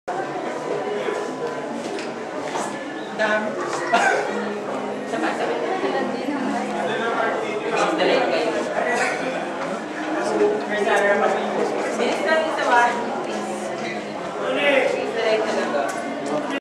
UNTAG/UNTOG exhibit (1)
ABMA 70's show their different talents.

This is held in CCP last saturday (May7, 2011)